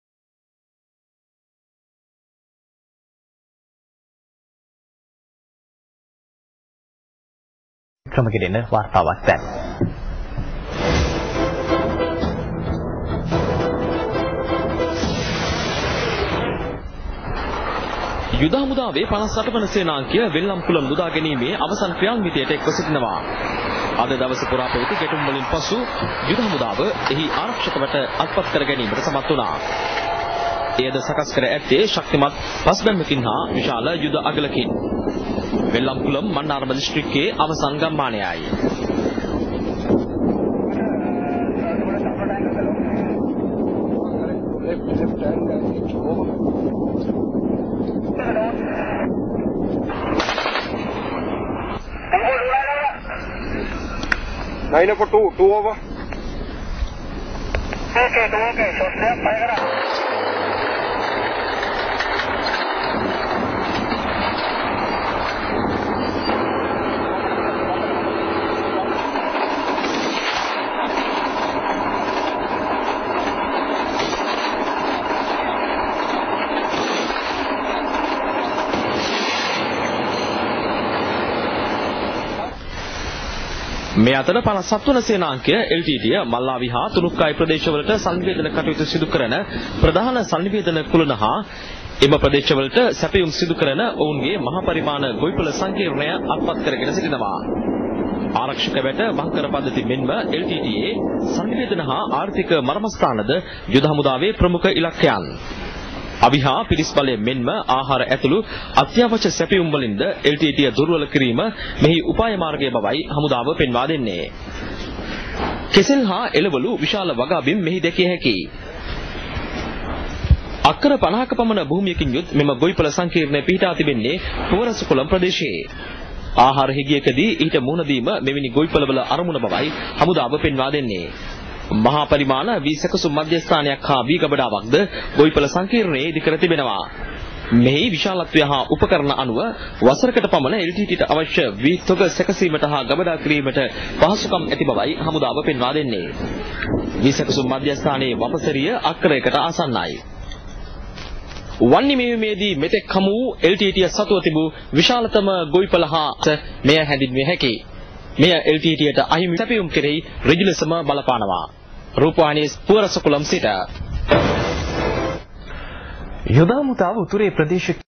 Wanni operation 29 th July 2008
Vellankulam liberation is at the final stage.
Wanni operation 29 th July 2008 (News report of Wanni meheyuma from SLRC)  PLease have look more information singhalaya.blogspot.com